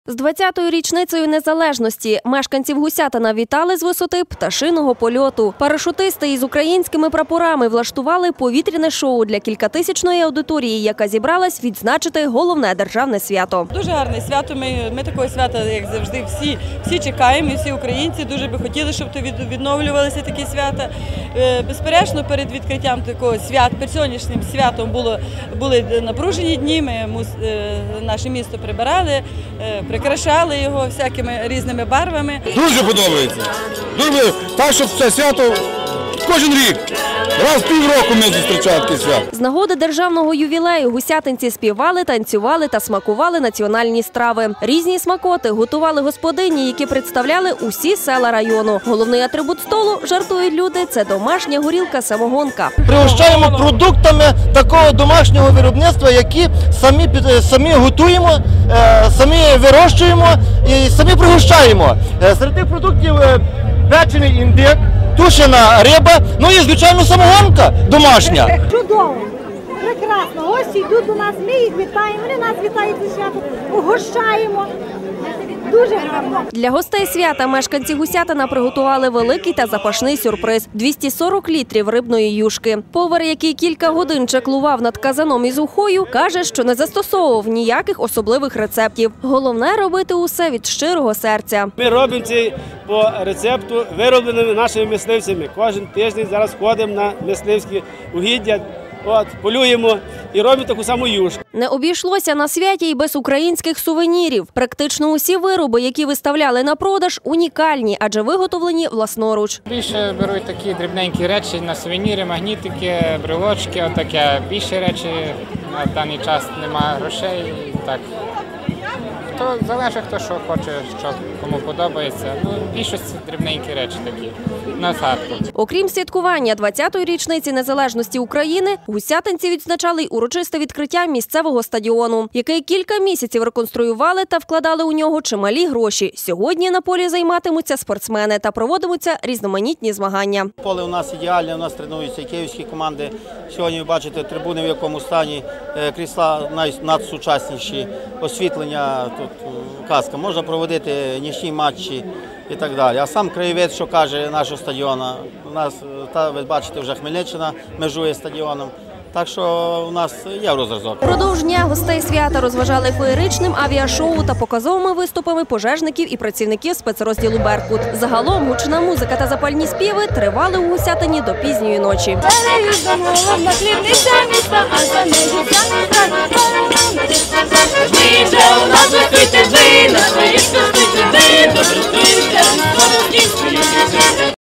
День незалежності у Гусятині відзначили раніше
З 20-ю річницею Незалежності мешканців Гусятина вітали з висоти «пташиного польоту». Парашутисти з українськими прапорами влаштували повітряне шоу для кількатисячної аудиторії, яка зібралась відзначити головне державне свято.
Сюжет телеканалу ІНТБ (м. Тернопіль). Ефір 22 серпня 2011 року.